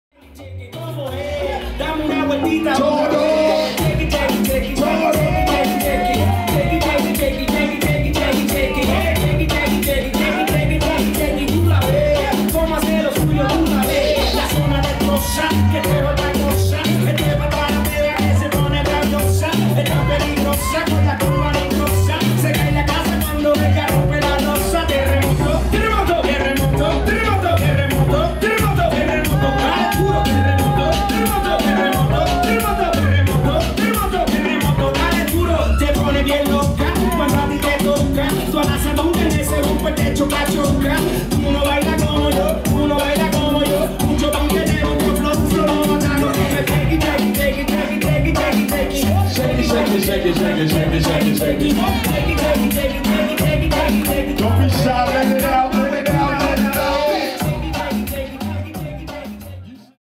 THE FLOOR Improv Night 114 - October 23rd 2017
THE FLOOR Improv Night 114 - 10:05 MASH UP - October 23rd  with Musical Lead The Red Line Horns Live at Los Globos. Video By Robertino Fonseca